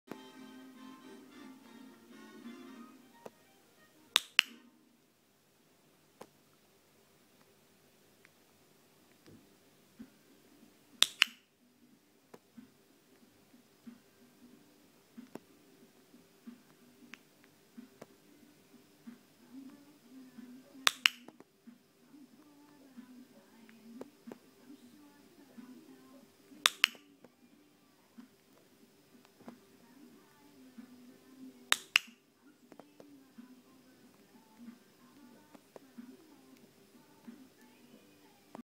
Eve: Lovebird Clicker Training
Eve seems to begin to understand click means a treat. This is a very short video showing her first shaping. Clikck for approaching the toy.
やっとクリッカーの意味がわかりかけてきた様子のイブぼったん。クリックの後、トリーツを渡す手を探すようになりました。ほんの短いビデオで質も良くありませんが、1人あいぽん撮影なのでお許しを。